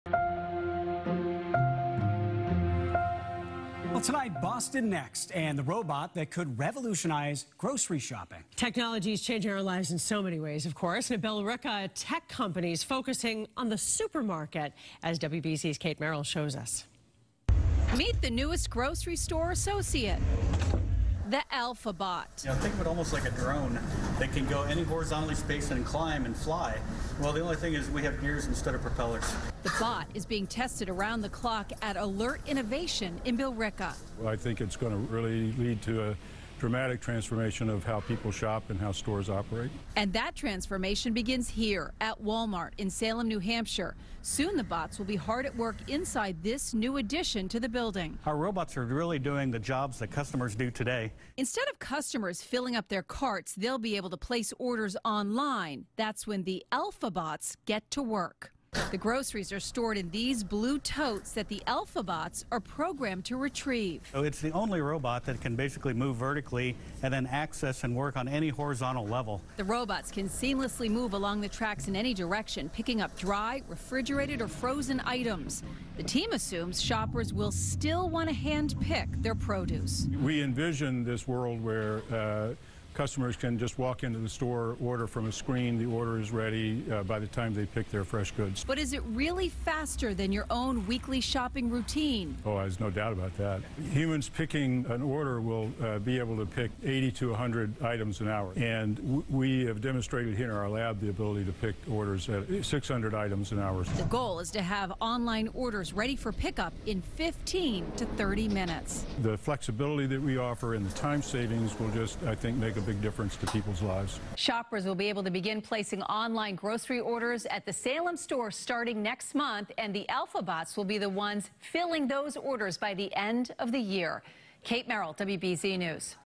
The Alphabot Aims To Transform How People Shop For Groceries
Meet the newest grocery store associate - the Alphabot. WBZ-TV's Kate Merrill reports.